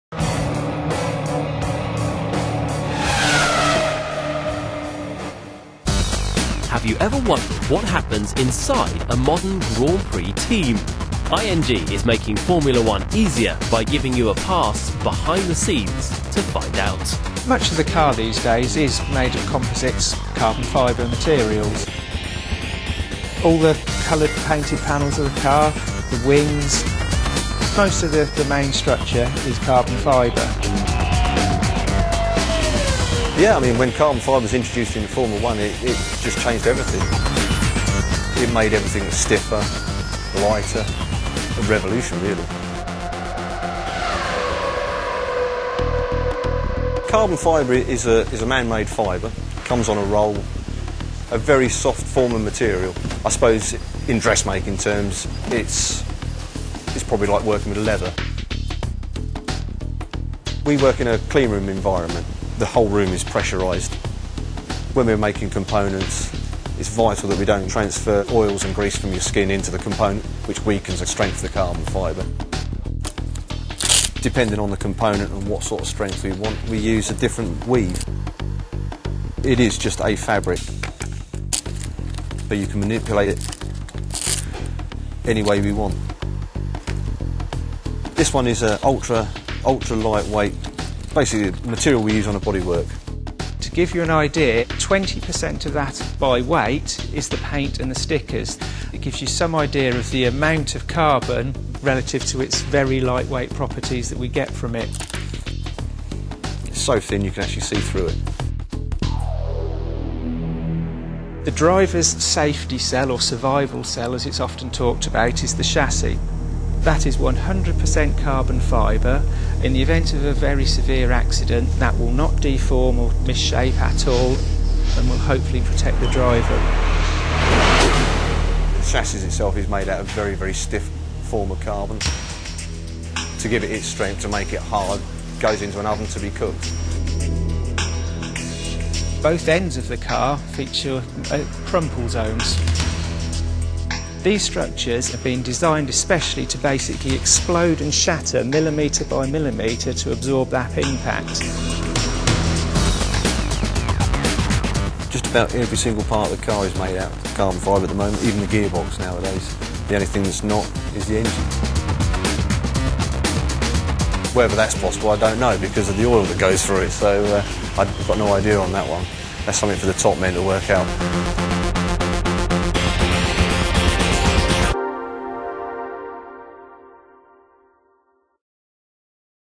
ING F1: Carbon Fibre explained
Many race fans don't know how or why a modern F1 car is constructed of carbon fibre -- even the steering wheel is made from it. This film looks at the reasons why, and follows the production process, from the carbon fibre sheet being cut with a pair of scissors in a sealed room, right through to Fernando Alonso driving out of the pit garage cocooned in a carbon fibre safety cell designed to withstand the force of a 200mph crash.

Interviews with:

Ian Goddard, Senior Composites Engineer, ING Renault F1 Team

Guy Rowles, Clean Room head, ING Renault F1 Team